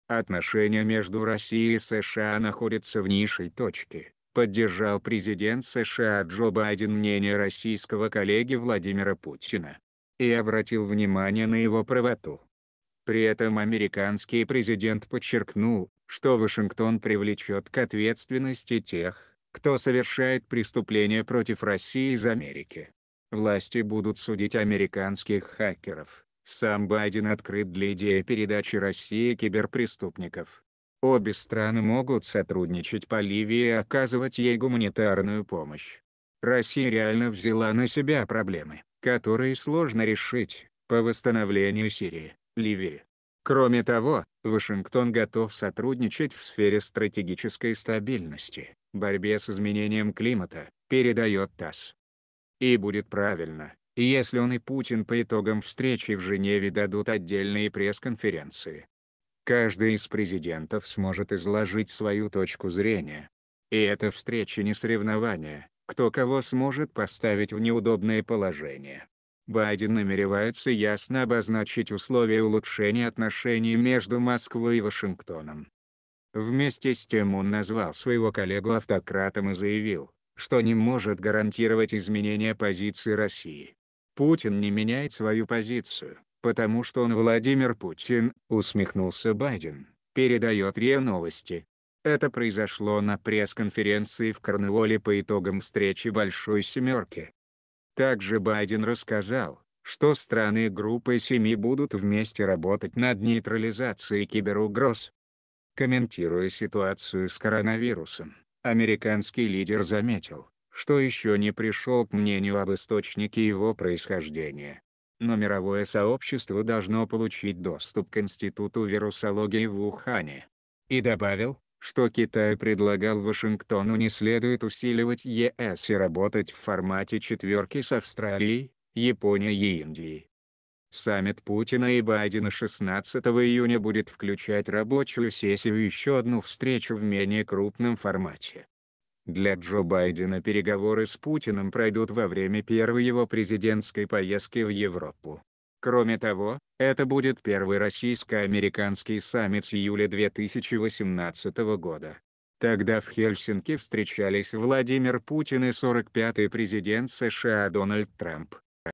Байден считает, что Путин прав
Президент США Джо Байден согласился с оценкой президента России Владимира Путина, что отношения между Россией и США находятся на низкой точке.